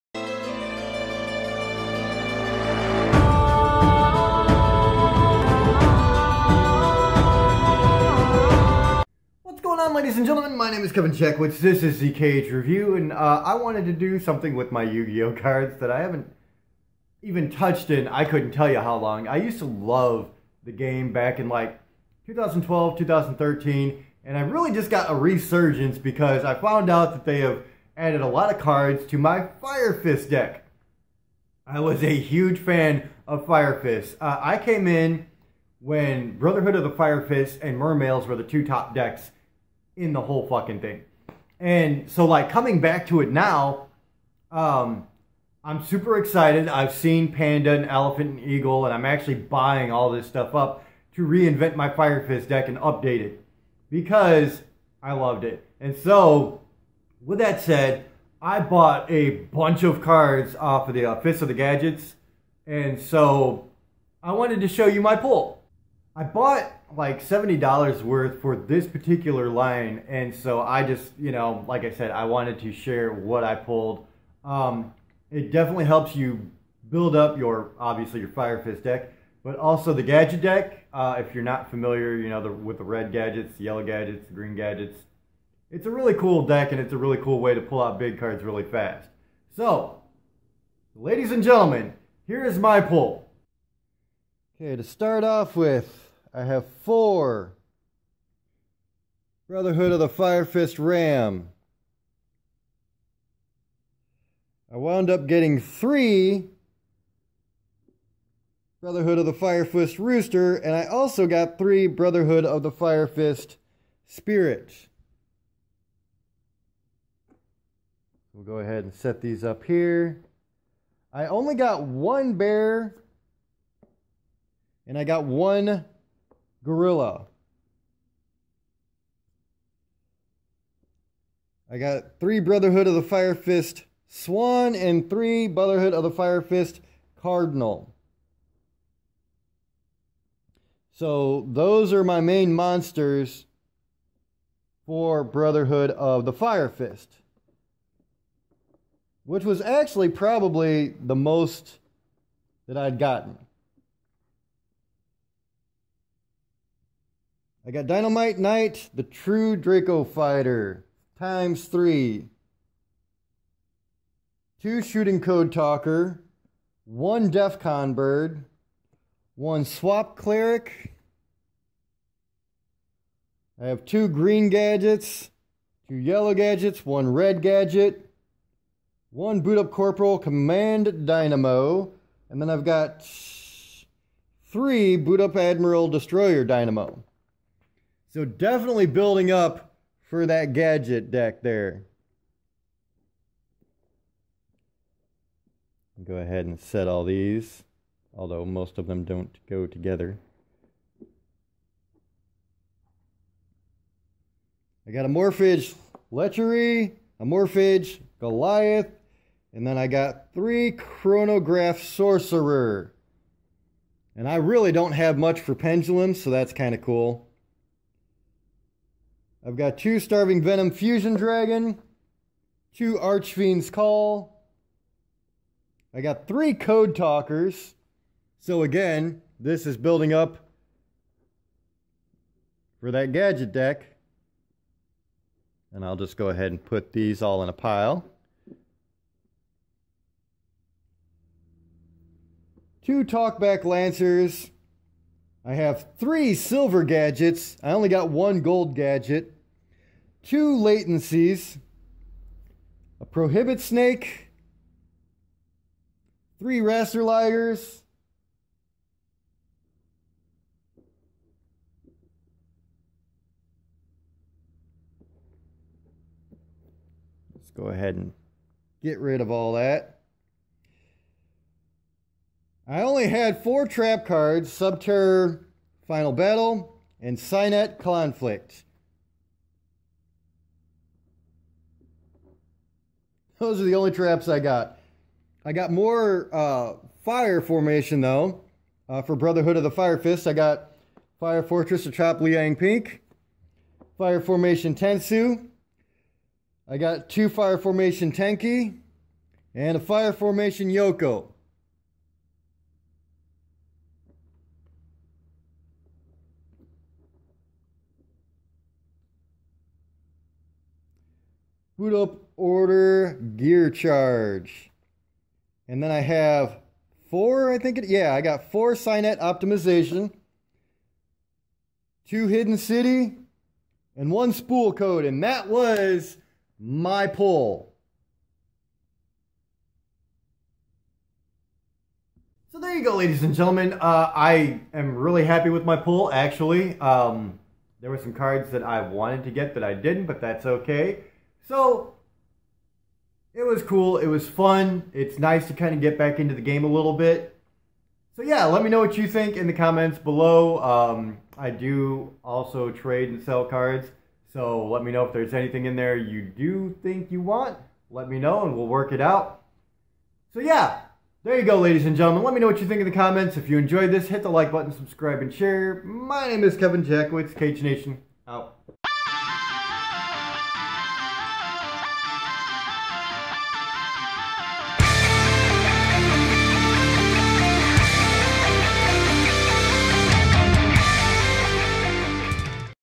My Pull from $75 00 Worth of Yu Gi Oh Fists of the Gadgets
I decided to show my pull from quite a few Yu-Gi-Oh Fists of the Gadgets booster packs. It's been quite some time since I played but I want to re-build my Brotherhood of the Fire Fists deck!